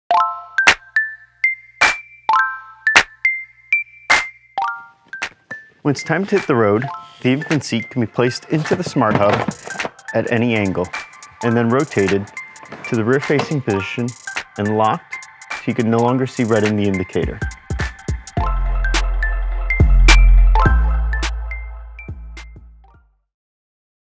Dock Your G5 Infant Car Seat Into Base
With babies, when it’s time to go, it’s really time to go. So you’re armed with the knowledge when it counts, here’s how to dock your Orbit Baby G5 Infant Car Seat into its SmartHub® Car Seat Base. The best part? This Orbit Baby Infant Car Seat is designed to dock at any angle, and then rotate into place, so it can save parents from injury, with a struggle-free docking experience.